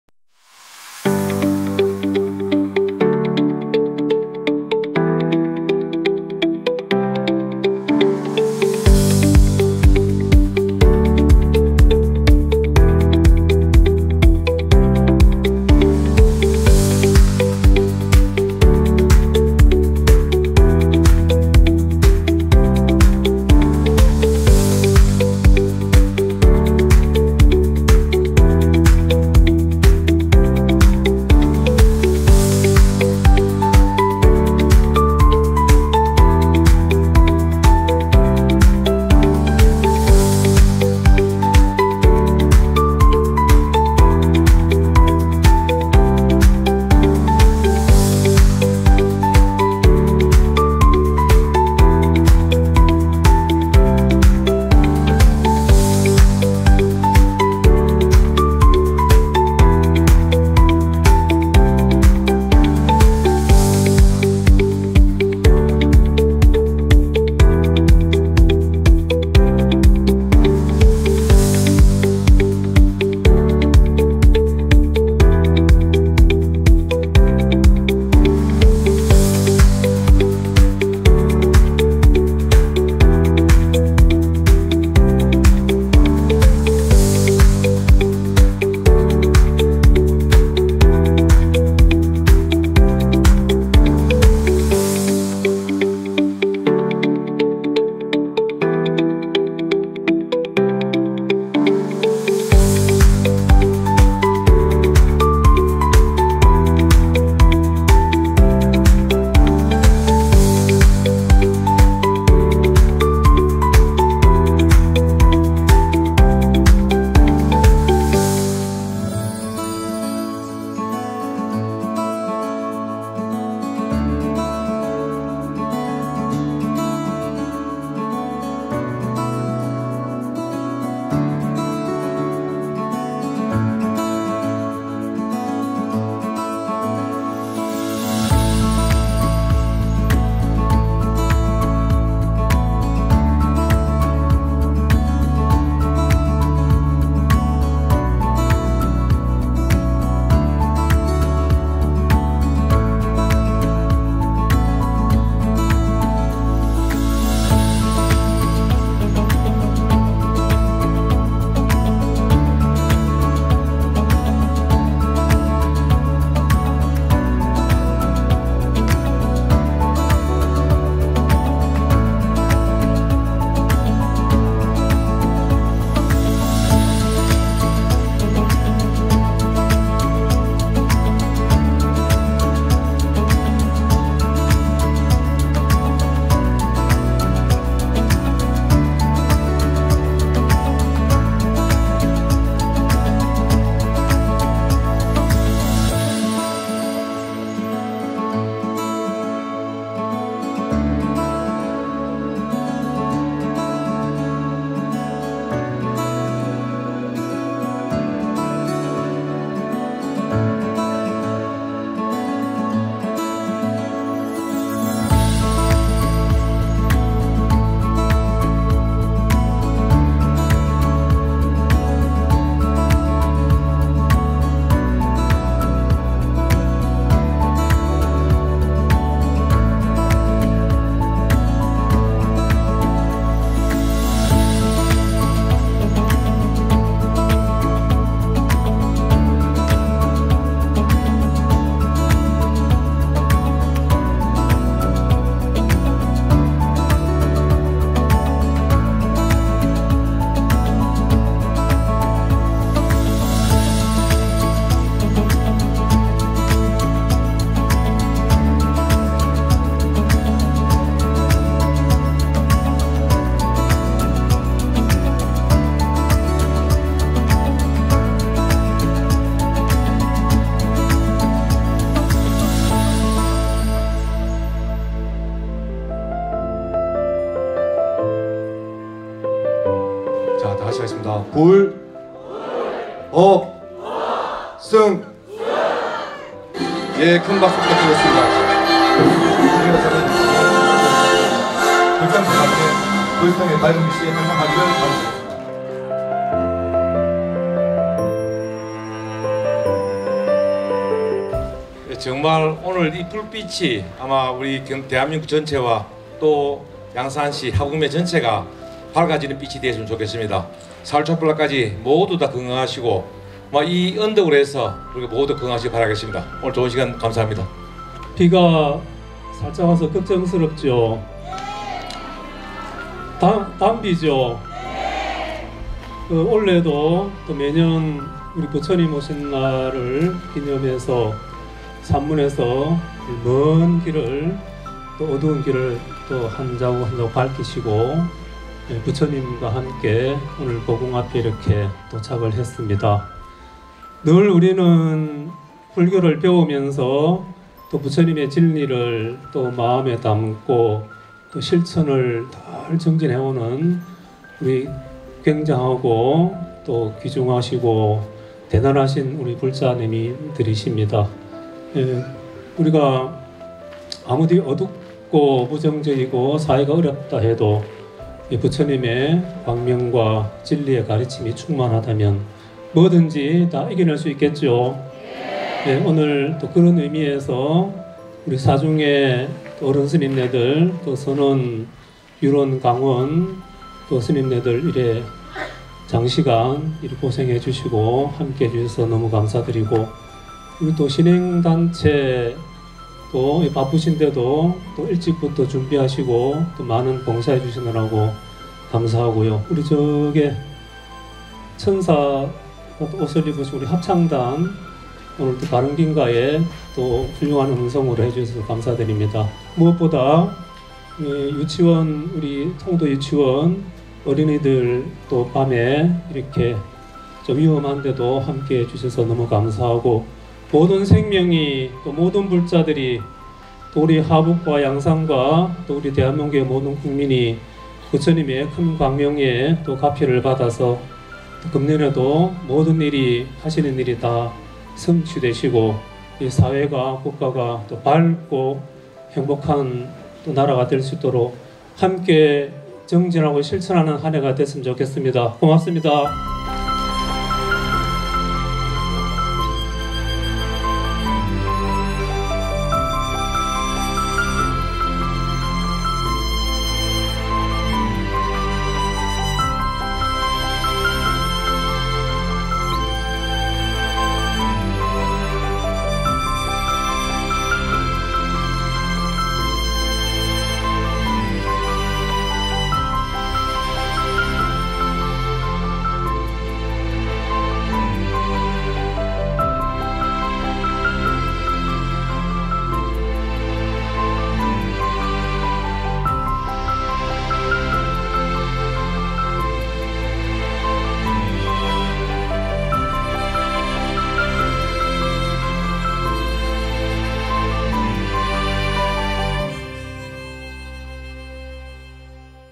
부처님오신날 맞이 통도사 제등행렬
불기2568년 부처님 오신날을 맞아 지난 5월 11일(토) 영축총림 통도사 사부대중들이 제등행렬을 거행하였습니다.
이 날 행렬에는 통도사 대중스님들과 신행단체를 비롯하여 많은 불자님들께서 동참하여 성대하로 환희롭게 행렬이 진행되었습니다.